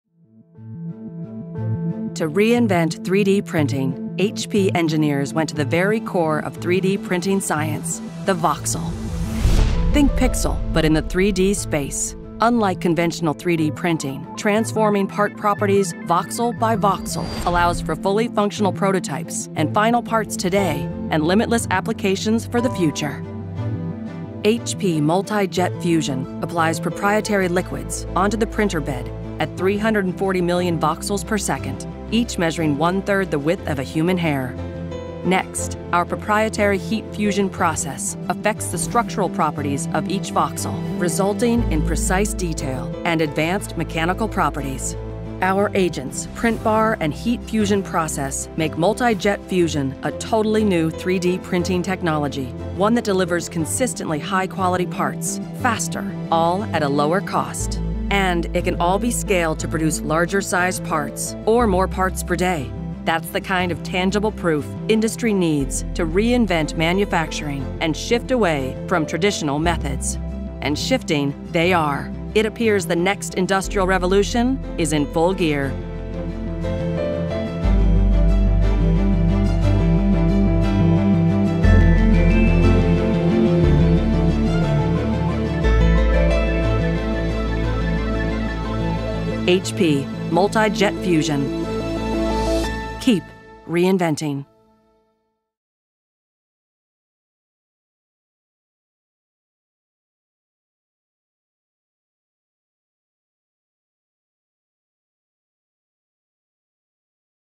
Reinventing 3D Printing | HP Multi Jet Fusion
To reinvent 3D printing, HP engineers went to the very core of 3D printing science - the voxel. Think pixel, but in a 3D space. Unlike conventional 3D printing, transforming part properties voxel by voxel allows for fully functional prototypes and final parts today and limitless applications for the future.

HP Multi Jet Fusion applies proprietary liquids onto the printer bed at 340 million voxels/second, each measuring 1/3 the width of a human hair. Next, our proprietary heat fusion process affects the structural properties of each voxel, resulting in precise detail and advanced mechanical properties. Our agents, print bar and heat fusion process make Multi Jet Fusion a totally new 3D printing technology. One that delivers consistently high quality parts faster, all at a lower cost. And it can all be scaled to produce larger size parts or more parts per day. That's the kind of tangible proof industry needs to reinvent manufacturing and shift away from traditional methods - and shifting they are.

It appears the next industrial revolution is in full gear.

""For our future roadmap, we see major potential in our partnership with HP."" - BMW

""Excited to partner with HP to accelerate performance products to help athletes reach their full potential"" - Nike

""The most disruptive event in manufacturing since the invention of 3D printing."" - Engineering.com

LEGAL FOOTNOTES:

Superior, consistent quality: Based on dimensional accuracy of ±0.2 mm/0.008 inches, measured after sand blasting and on the following mechanical properties: Tensile strength at 45-50 Mpa (XYZ), Modulus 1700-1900 Mpa (XYZ). ASTM standard tests with PA12 material.

Up to 10 times faster/Breakthrough Productivity: Based on internal testing and simulation, HP Jet Fusion 3D printing solution average printing time is up to 10x faster than FDM & SLS printer solutions from $100,000 USD to $300,000 USD on market as of April 2016. Testing variables: Part Quantity -1 full build chamber of parts from HP Jet Fusion 3D at 20% of packing density vs same number of parts on above-mentioned competitive devices; Par tsize: 30g; Layer thickness: 0.1 mm/0.04 inches. Fast Cooling is enabled by HP Jet Fusion 3D Processing Station with Fast Cooling, available in 2017. HP Jet Fusion 3D Processing Station with Fast Cooling accelerates parts cooling time vs recommended manufacturer time of SLS printer solutions from $100,000 to $300,000 USD, as tested in April 2016. FDM not applicable.

Continuous printing: Continuous printing requires an additional HP Jet Fusion 3D Build Unit (standard printer configuration includes one HP Jet Fusion 3D Build Unit)

Lowest Cost-Per-Part: Based on internal testing and public data, HP Jet Fusion 3D printing solution average printing cost-per-part is half the cost of comparable FDM & SLS printer solutions from $100,000 USD to $300,000 USD on market as of April 2016. Cost analysis based on: standard solution configuration price, supplies price, and maintenance costs recommended by manufacturer. Cost criteria: printing 1-2 build chambers per day/5 days per week over 1 year of 30-gram parts at 10% packing density using the powder reusability ratio recommended by manufacturer.

Cleaner workflow: The term ""cleaner"", does not refer to any indoor air quality requirements and/or consider related air quality regulations or testing that may be applicable. The HP powder and agents do not meet the criteria for classification as hazardous according to Regulation (EC) 1272/2008 as amended.

HP 3D High Reusability PA12: HP Jet Fusion 3D print solution with HP 3D High Reusability PA12 has the highest post-production surplus powder reusability with 80% reusability vs any other powder based 3D printing technology using PA12 material. Consistent performance with only 20% powder refresh rate.